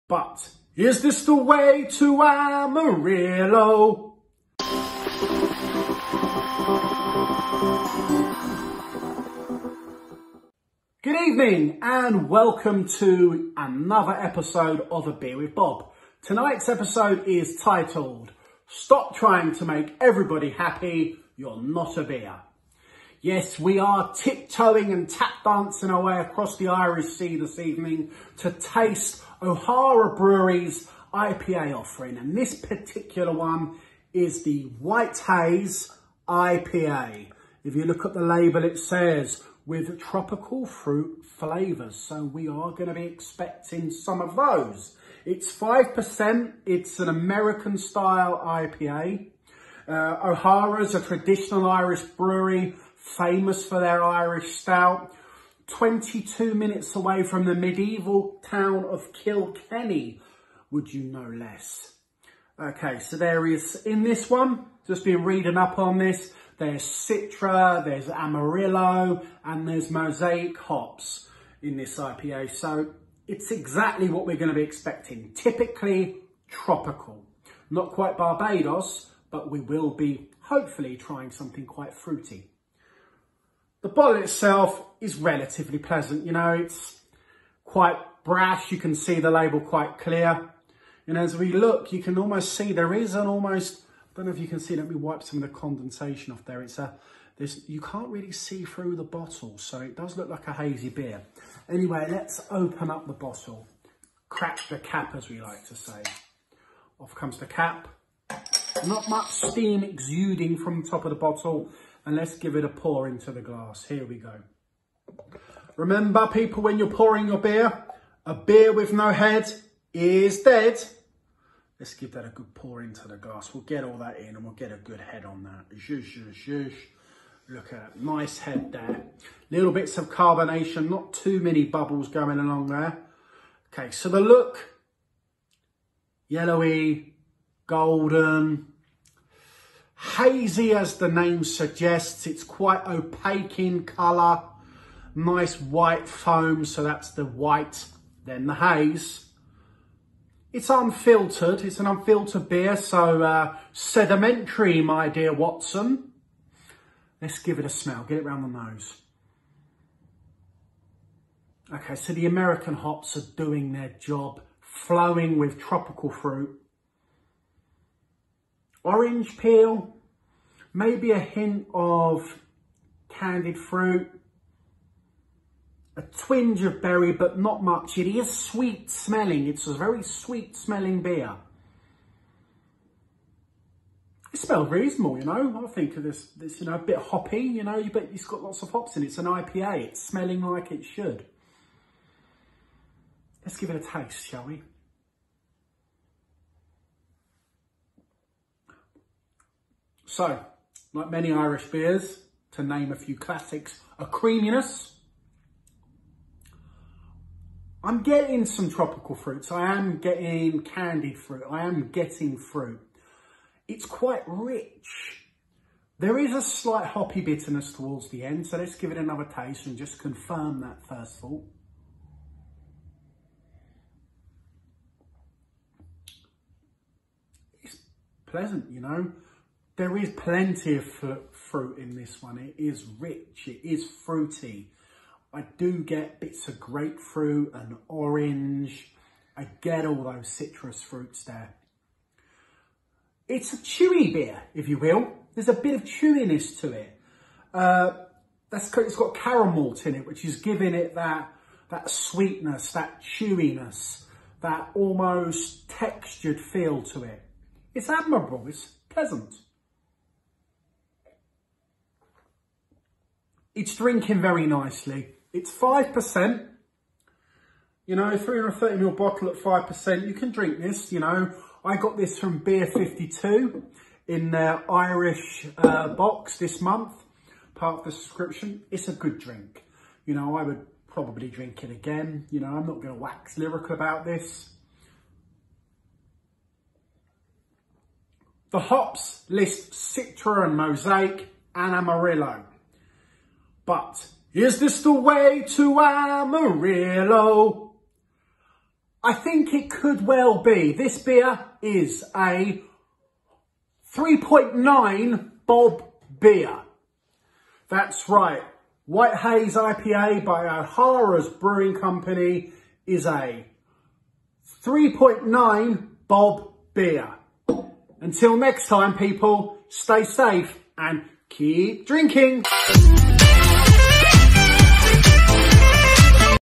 A Beer With Bob Ep.11 - O'Hara's White Haze IPA
Episode 11 of a Beer with Bob!

A fine beer today as bob dives deep into explaining the tastes and deliciousness of this fine beer!